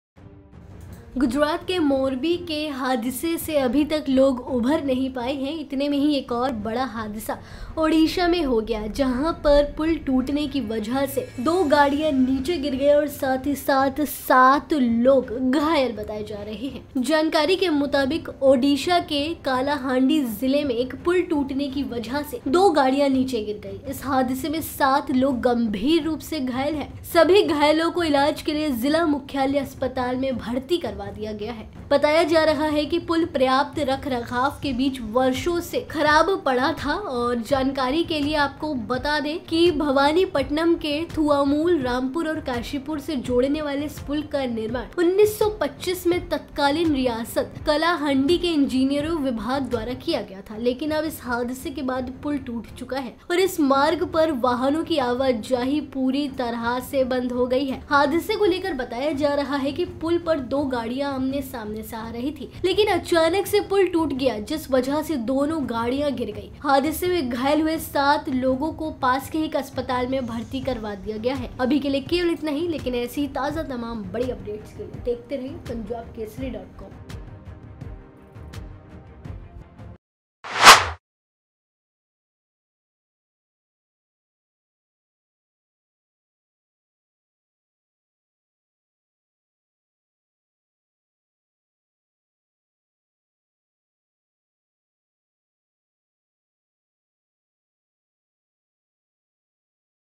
गुजरात की तरह ओडिशा में भी टूटा पुल, 2 गाड़ियां पुल से नीचे गिरी, 7 लोग गंभीर रूप से घायल
Two vehicles fell down after a bridge collapsed on Bhawanipatna-Thuamul Rampur road in Odisha's Kalahandi district. Seven people were seriously injured in this accident. All the injured have been taken to the district headquarters hospital for treatment. The bridge is said to have been dilapidated over the years without adequate maintenance